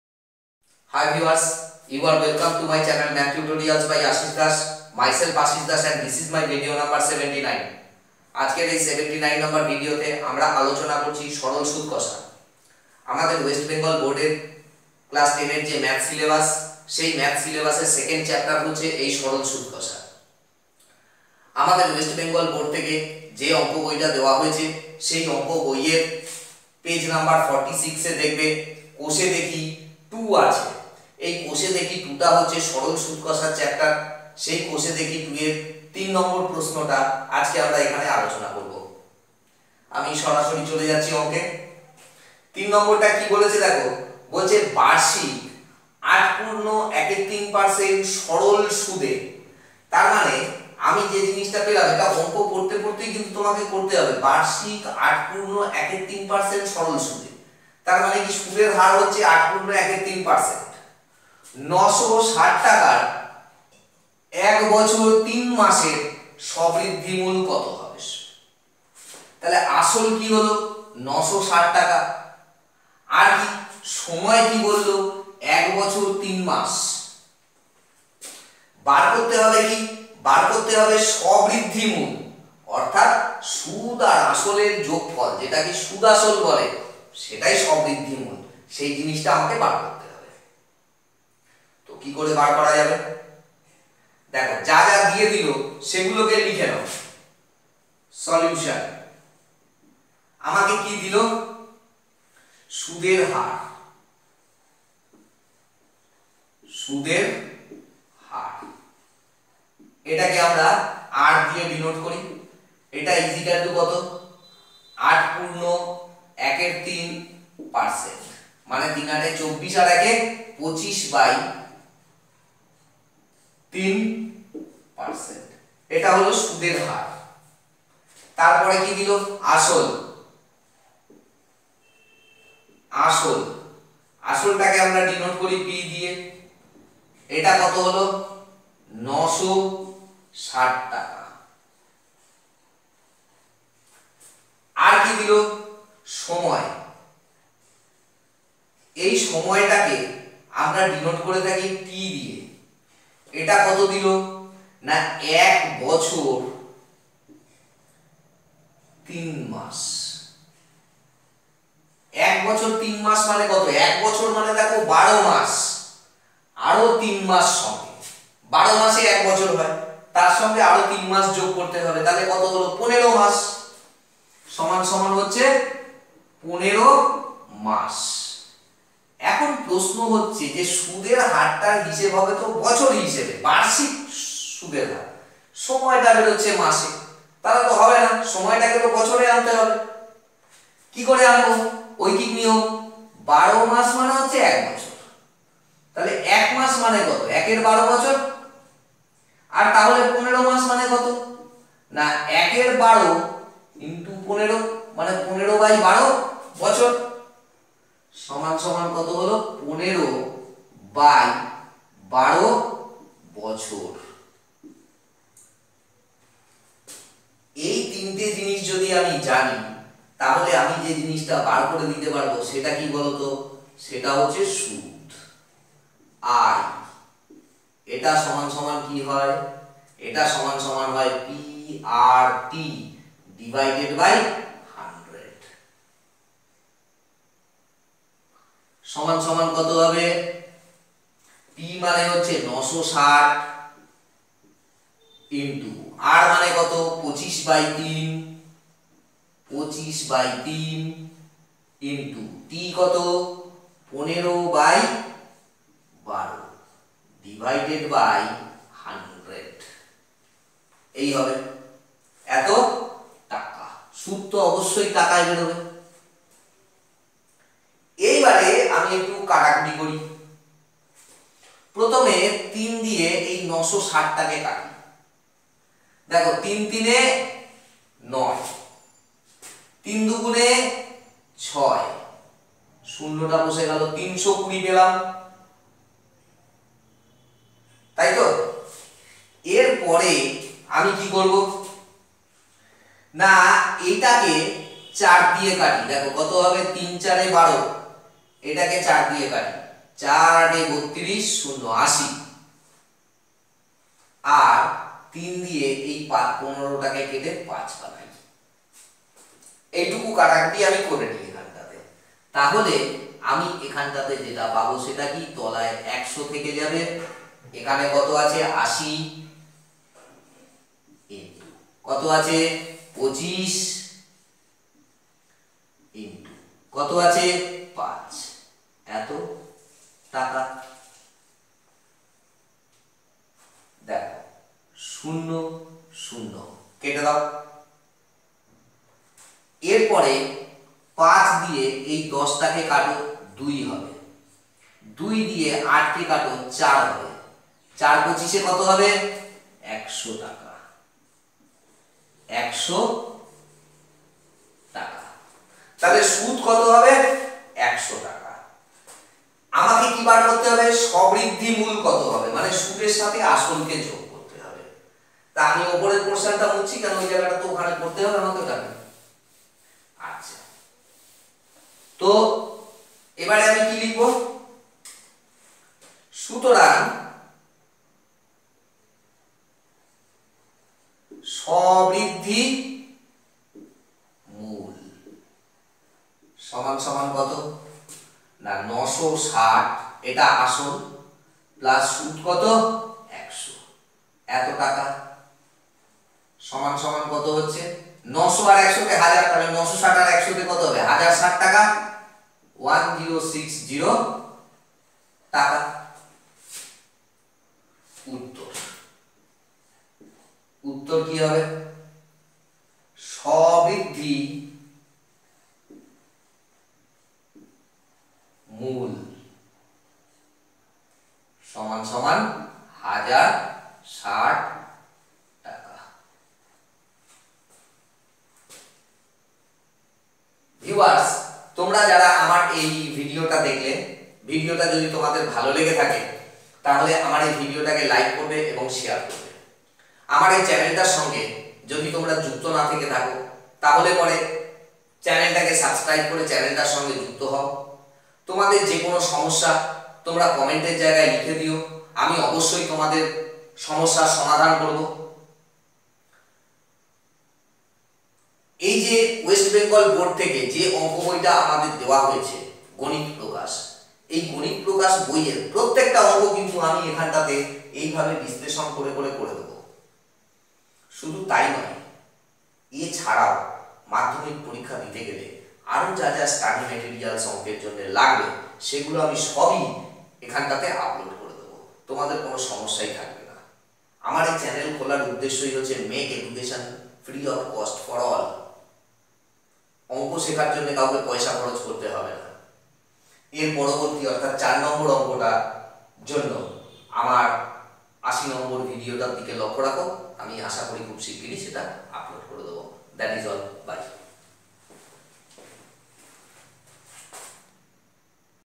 Video-79, Sraral Sudkasha(Simple Interest) Madhyamik Maths for Class 10 of West Bengal Board(WBBSE)
Saral Sudkasha(Simple interest) Maths For Class 10 Of Madhyamik Examination Of West Bengal Board(WBBSE)  in Bengali. Ganit Prakash Math Book, Question No.-3